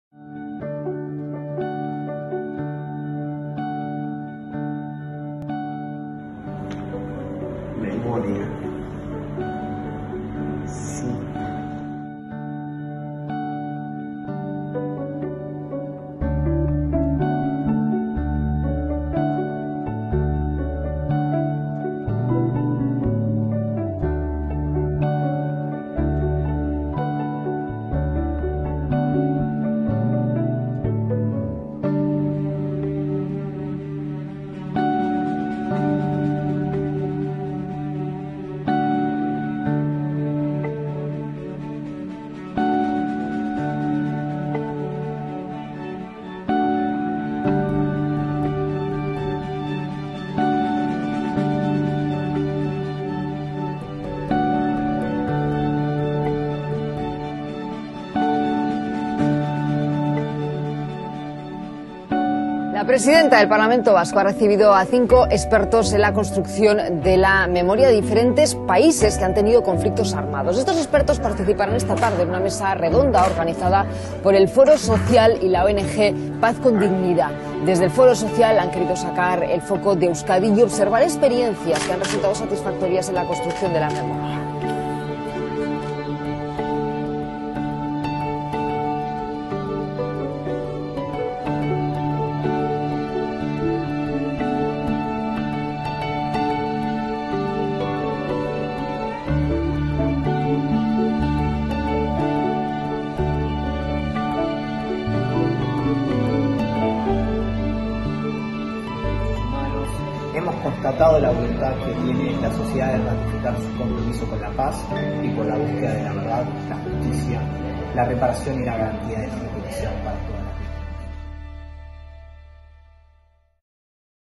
Hitzorduak: Memoria crítica inclusiva. Un aporte para la convivencia democrática. Vídeo resumen.
2020/02/20 - Visita de expertas en derechos humanos al País Vasco, invitadas por el Foro Social Permanente y Paz con Dignidad.
Han participado Ndeye Marie Sagna Le Caer (Senegal, Coordinadora de la Asociación de Mujeres por la construcción de la Paz en Casamance - Kabon Ketoor, Maritze Trigos Torres (Colombia, Monja dominica y Defensora de Derechos Humanos, Rodrigo Gomez Tortosa (Argentina), Abogado y Político, Secretario Ejecutivo de la Coalición Internacional contra las Desapariciones Forzosas y Gustavo Meoño Brenner (Guatemala), Ex director del Archivo Histórico de la Policía Nacional de Guatemala.